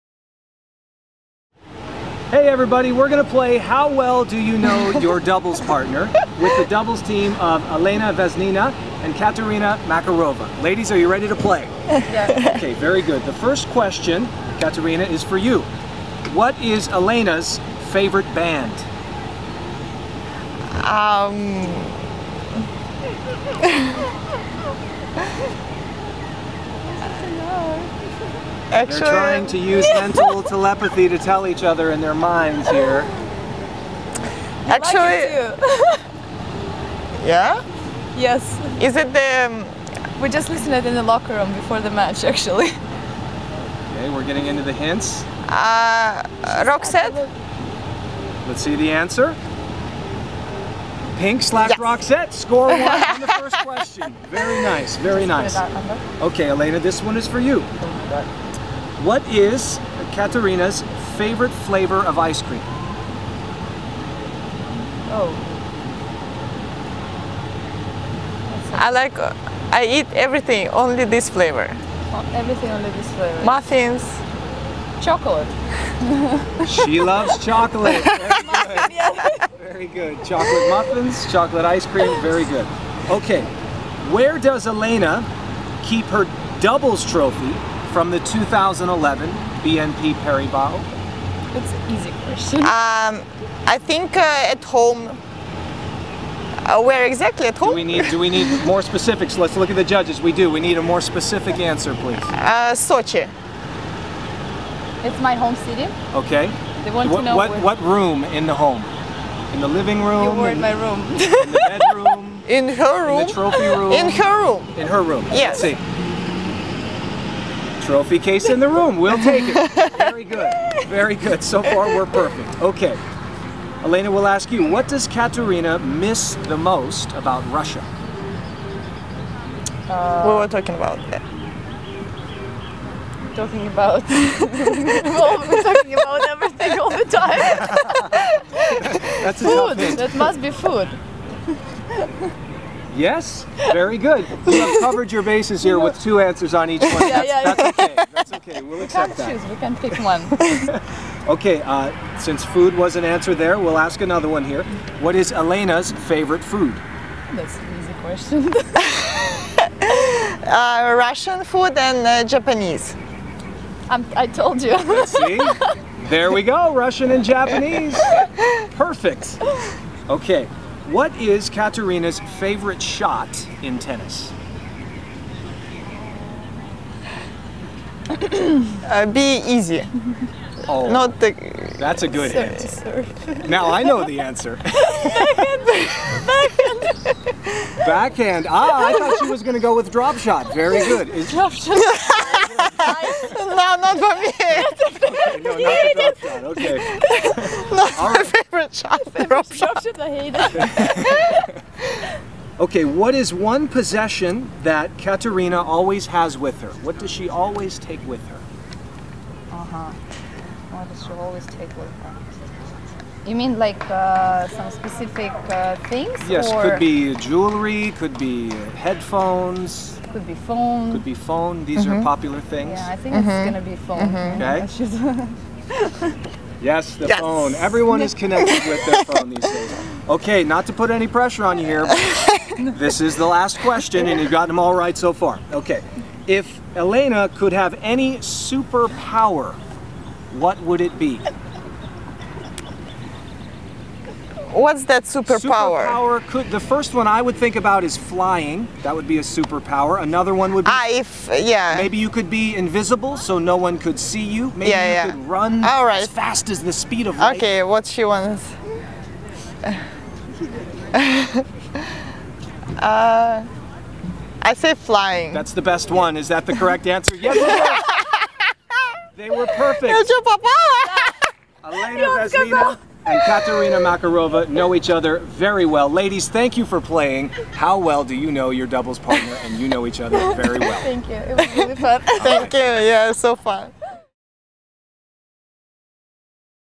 BNP Paribas Open - How Well Do You Know... Makarova and Vesnina
The No. 4 seeds at the BNP Paribas Open, Elena Vesnina and Ekaterina Makarova are quizzed on how well they know each other.